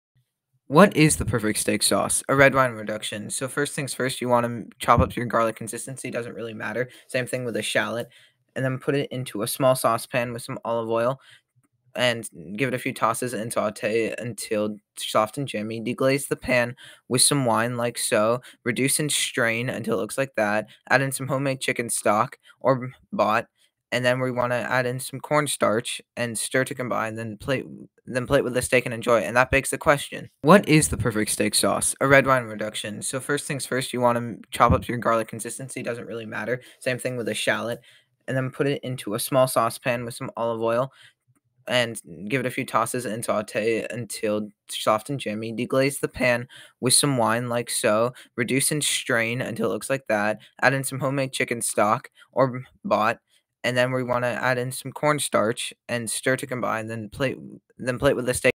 The PERFECT Steak Sauce (Red Wine Reduction)
Today I'm making the PERFECT steak sauce: A red wine reduction.
Ingredients:
-3 cloves of garlic
-1 Shallot
- 2 cups of red wine
- 1 cup of Chicken Stock
-1 sprig of thyme and rosemary